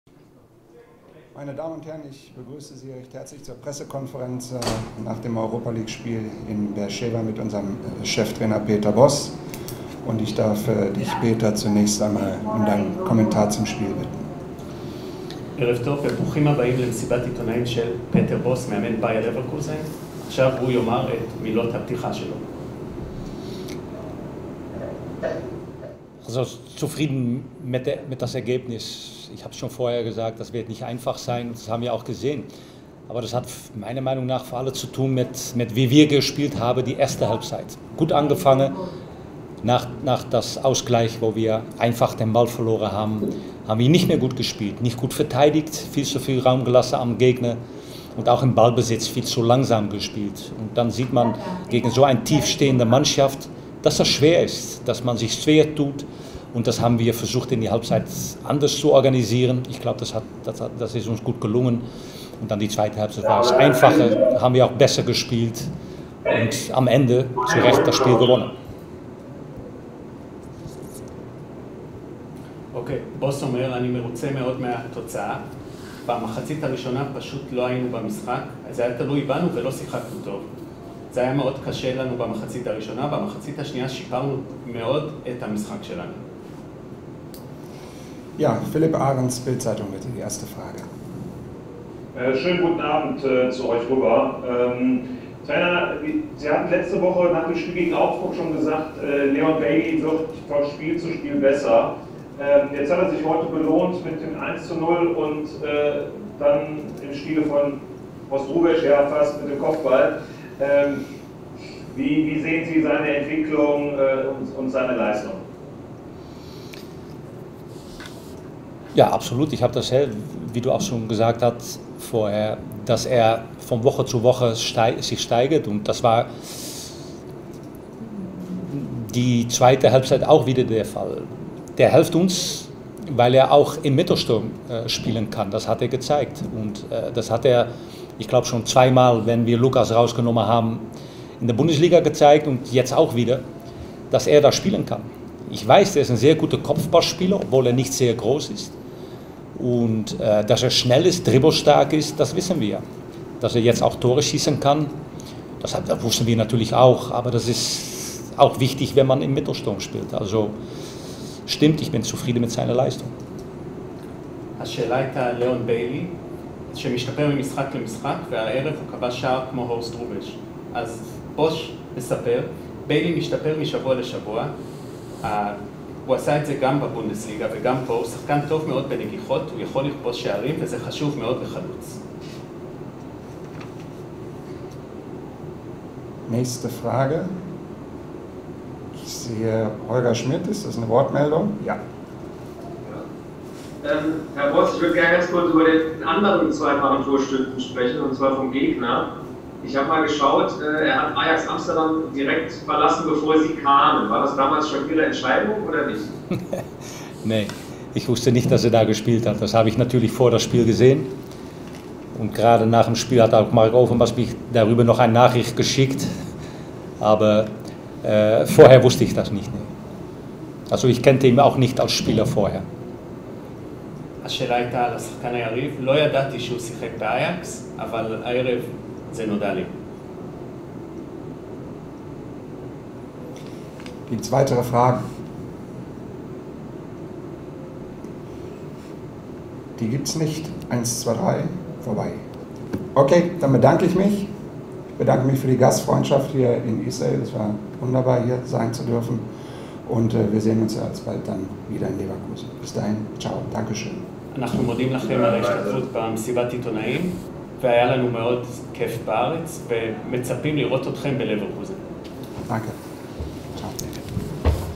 "Haben uns schwer getan" | PK mit Peter Bosz nach Hapoel Beer Sheva 🆚 Bayer 04 Leverkusen 2:4 | UEL
Bayer 04 Leverkusen hat auch dank eines Doppelpacks von Leon Bailey das Weiterkommen in der UEFA Europa League fest im Blick. Das nach kräftezehrenden Tagen runderneute Team von Peter Bosz gewann beim israelischen Pokalsieger Hapoel Be'er Scheva mit viel Mühe 4:2 (2:2) und hat nach drei Spielen sechs Punkte auf dem Konto. Bailey (5., 75.), ein Eigentor von Or Dadia (39.) und Florian Wirtz (88.) sorgten für den Leverkusener Sieg. Für die Gastgeber war der starke Elton Acolatse (11., 25) doppelt erfolgreich.